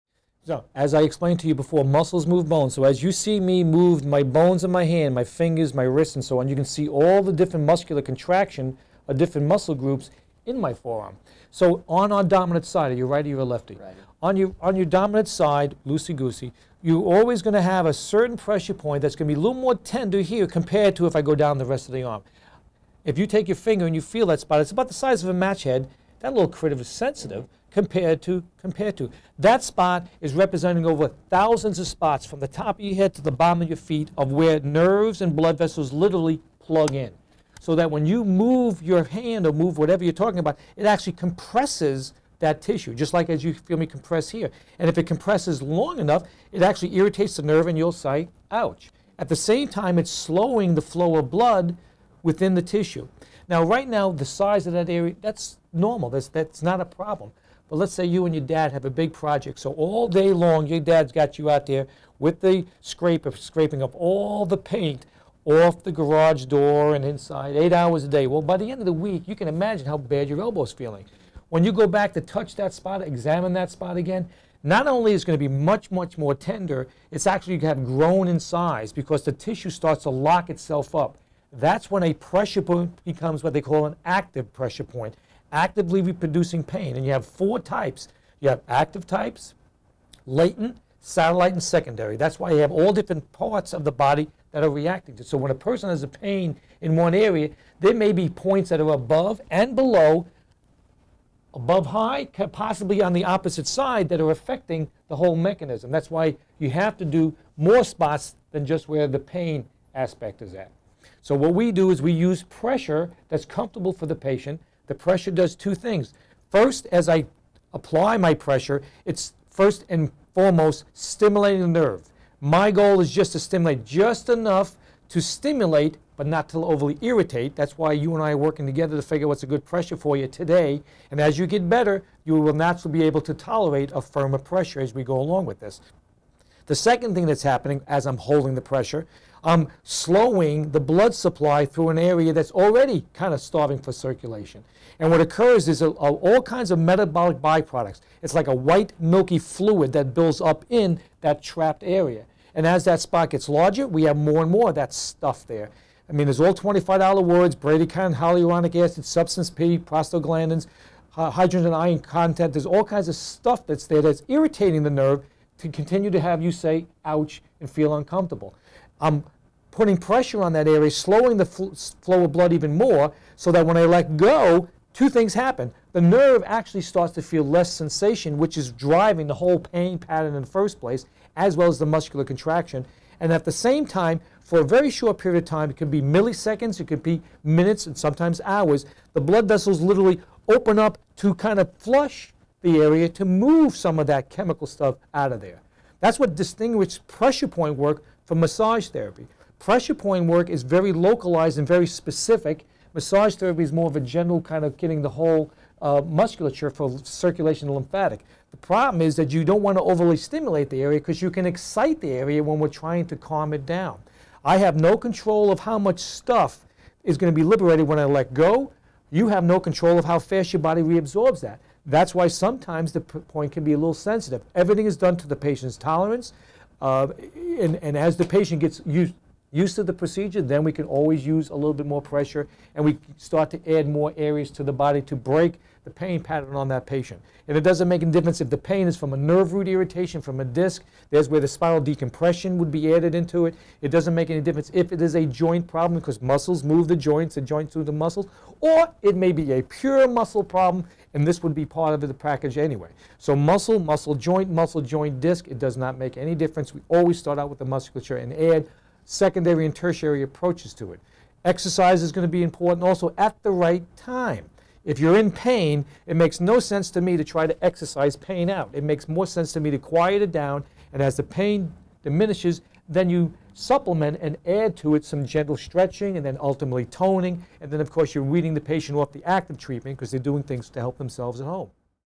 Explain Muscles
Demo and explanation of acupressure/myofascial and trigger points